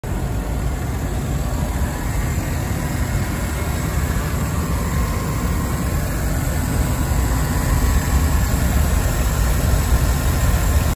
GovDeals: 2013 Chevrolet Tahoe Police
2013 Chevrolet Tahoe Police sold by Louisiana State Surplus, LA on GovDeals.com